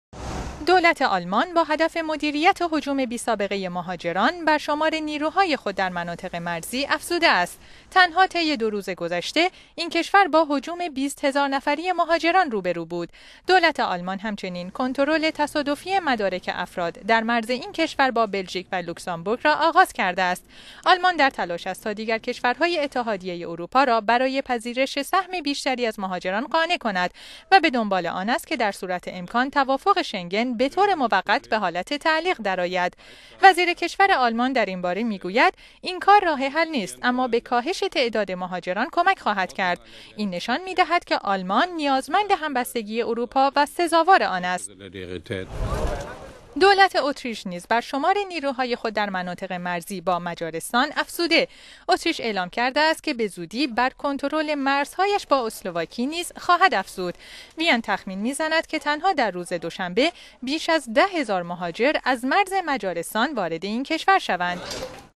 افزایش شمار نیروهای مرزی آلمان و اتریش برای کنترل هجوم مهاجران
دولت آلمان با هدف مدیریت هجوم بی سابقه مهاجران، بر شمار نیروهای خود در مناطق مرزی افزوده است. تنها طی دو روز گذشته، این کشور با هجوم ۲۰ هزارنفری مهاجران روبرو بود. دولت آلمان همچنین کنترل تصادفی مدارک افراد در مرز این کشور با بلژیک و لوکزامبورگ را آغاز کرده است.

آلمان در تلاش است تا دیگر کشورهای اتحادیه اروپا را برای پذیرش سهم بیشتری از مهاجران قانع کند و همچنین به دنبال آن است که در صورت امکان، توافق شنگن بطور موقت به حالت تعلیق درآید.

توماس دمیزر، وزیر کشور آلمان در این باره می گوید: «این، چ…